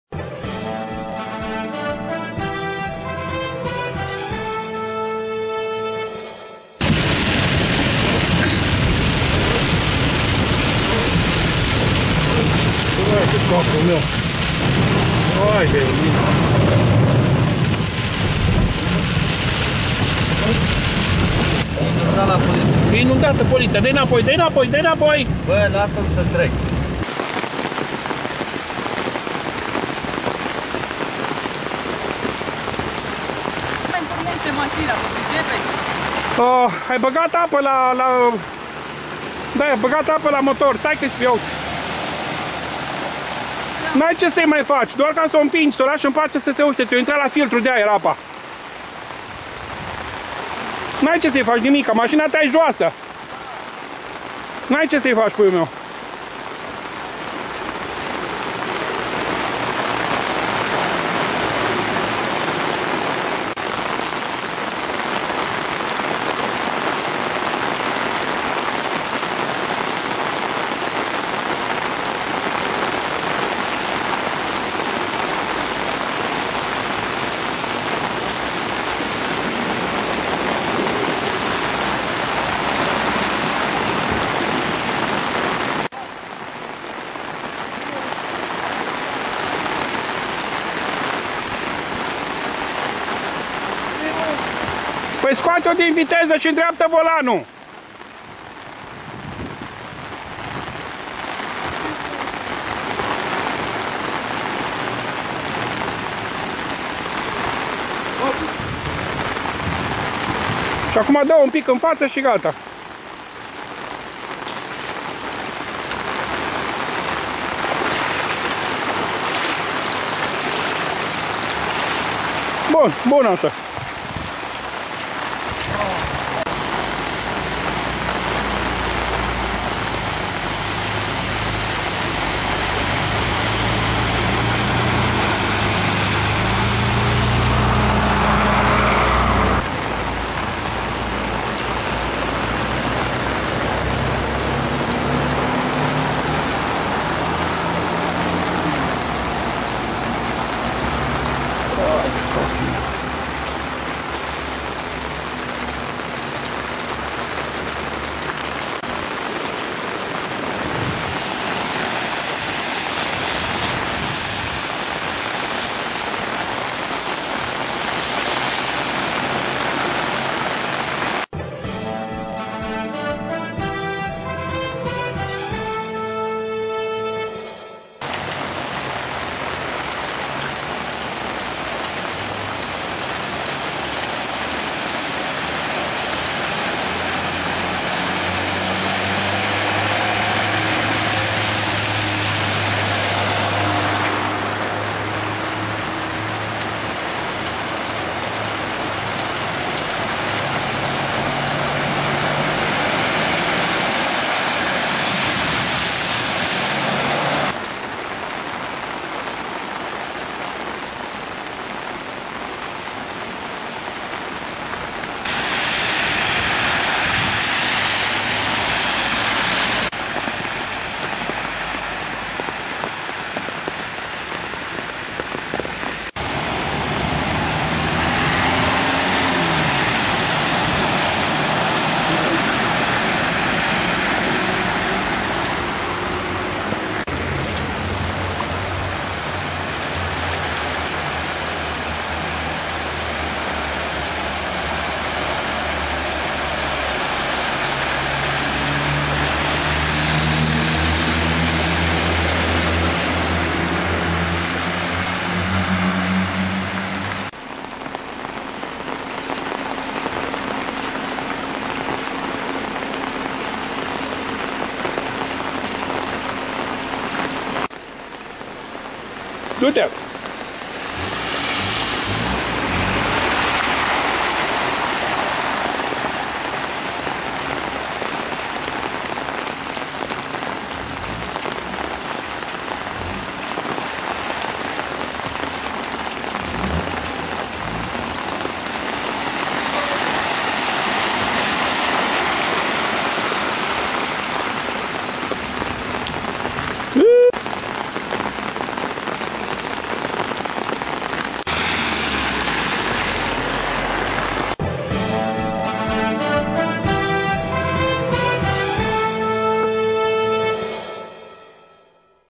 Ploaie torentiala cu inundatii la Resita 28 iun 2016
O ploaie torenţială  s-a abătut astă noapte asupra municipiului Reşiţa și peste Banatul Montan. Județul era deja sub cod galben de vreme rea, iar în jurul orei 23, ISU Semenic a emis și o avertizare de cod portocaliu imediat.  Ploaia căzută a transformat în câteva minute străzile oraşului în adevărate râuri. Canalele nu au putut prelua cantitatea imensă de apă ce s-a scurs din cer.
Din cauza apei, circulaţia a fost blocată  pe mai multe artere ale oraşului, mai exact, toate intersecțiile mai joase ale Reşiţei. În fața Școlii Generale nr.6 și în fața sediului ,,Poliției” - s-au format adevărate lacuri lungi de aproape 50- 60 de m și de o adâncime considerabilă astfel încât mai mulți șoferi neinspirați au rămas cu mașinile în mijlocul lacului. Alți șoferi care au parcate mașinile pe marginea drumului, au avut surpriza să le găsească pline cu apă.